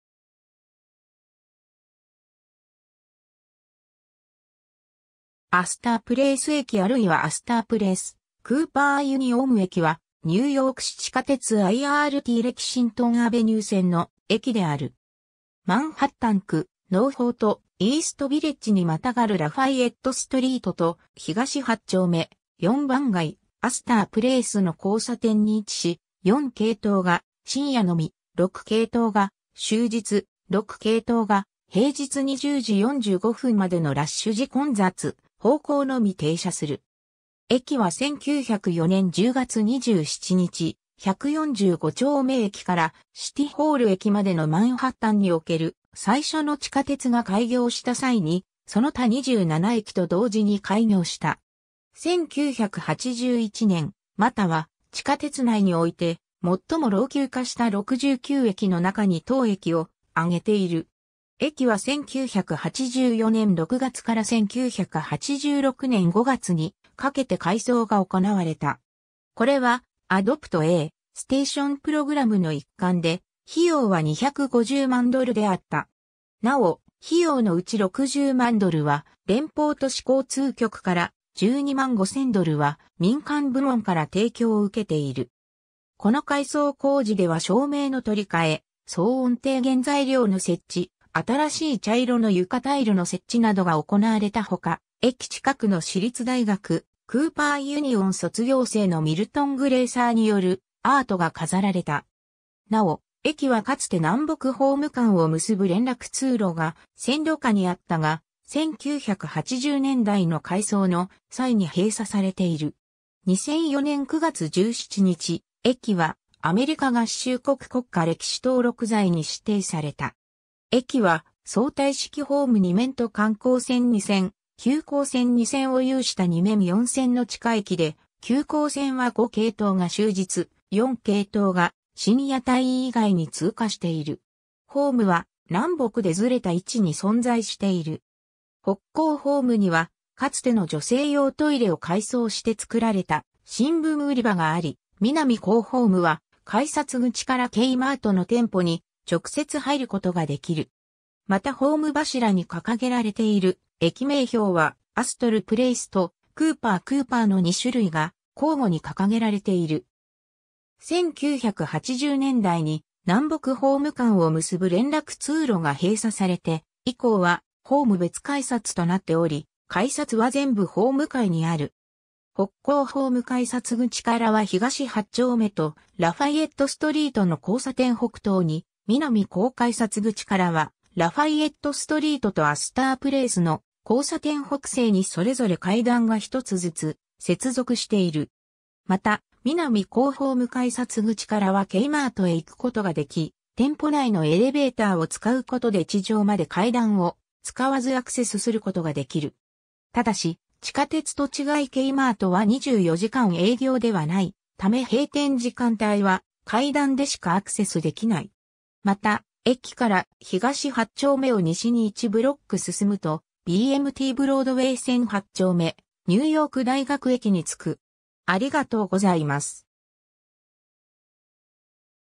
アスター・プレイス駅
IRTレキシントン・アベニュー線の鉄道駅
イースト・ヴィレッジ
アスター・プレイス駅 (Astor Place) あるいはアスター・プレイス-クーパー・ユニオン駅 (Astor Place–Cooper Union) はニューヨーク市地下鉄IRTレキシントン・アベニュー線の駅である。
マンハッタン区ノーホーとイースト・ヴィレッジに跨がるラファイエット・ストリートと東8丁目、4番街、アスター・プレイスの交差点に位置し、4系統が深夜のみ、6系統が終日、「6」系統が平日20時45分までのラッシュ時混雑方向のみ停車する。
 駅は1904年10月27日、145丁目駅からシティ・ホール駅までのマンハッタンにおける最初の地下鉄が開業した際にその他27駅と同時に開業した。
 1981年、MTAは地下鉄内において最も老朽化した69駅の中に当駅を挙げている。
駅は1984年6月から1986年5月に掛けて改装が行われた。
これはAdopt-a-Stationプログラムの一環で、費用は2,500,000ドルであった。
なお、費用のうち600,000ドルは連邦都市交通局 (Federal Urban Mass Transit Administration) から、125,000ドルは民間部門から提供を受けている。
この改装工事では照明の取り替え、騒音低減材料の設置、新しい茶色の床タイルの設置などが行われたほか、駅近くの私立大学クーパー・ユニオン卒業生のミルトン・グレイサーによるアートが飾られた。
なお、駅はかつて南北ホーム間を結ぶ連絡通路が線路下にあったが、1980年代の改装の際に閉鎖されている。
 2004年9月17日、駅はアメリカ合衆国国家歴史登録財に指定された。
 駅は相対式ホーム2面と緩行線2線・急行線2線を有した2面4線の地下駅で、急行線は5系統が終日、4系統が深夜帯以外に通過している。
 ホームは南北でずれた位置に存在している。
北行ホームにはかつての女性用トイレを改装して作られた新聞売り場があり、南行ホームは改札口からKmartの店舗に直接入る事ができる。
またホーム柱に掲げられている駅名標は"Astor Place"と"Cooper Cooper"の2種類が交互に掲げられている。
 1980年代に南北ホーム間を結ぶ連絡通路が閉鎖されて以降はホーム別改札となっており、改札は全部ホーム階にある。
北行ホーム改札口からは東8丁目とラファイエット・ストリートの交差点北東に、南行改札口からはラファイエット・ストリートとアスター・プレイスの交差点北西にそれぞれ階段が1つずつ接続している。
また、南行ホーム改札口からはKmartへ行くことができ、店舗内のエレベーターを使うことで地上まで階段を使わずアクセスすることができる。
ただし、地下鉄と違いKmartは24時間営業では無いため閉店時間帯は階段でしかアクセスできない。
 また、駅から東8丁目を西に1ブロック進むとBMTブロードウェイ線8丁目-ニューヨーク大学駅に着く。